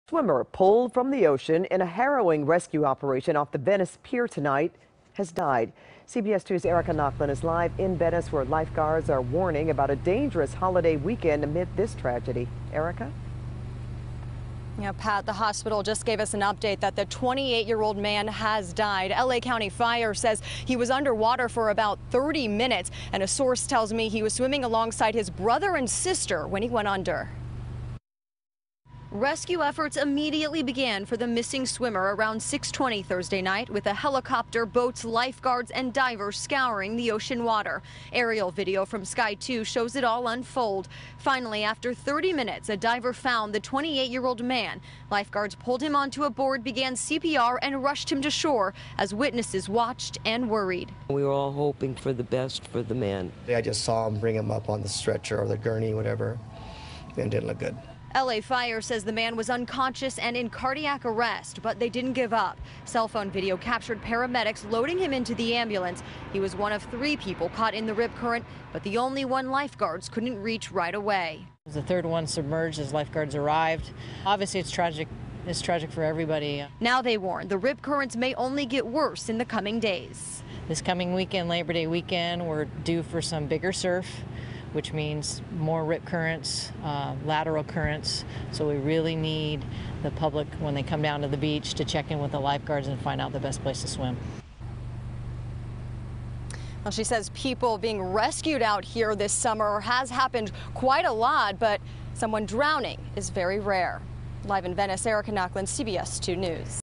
Swimmer Pulled From Ocean Off Venice Dies
A 28-year-old man who was transported to the hospital in grave condition Thursday night after he was pulled from the water near the Venice Pier by lifeguards was pronounced dead at the hospital. Erika Nochlin reports.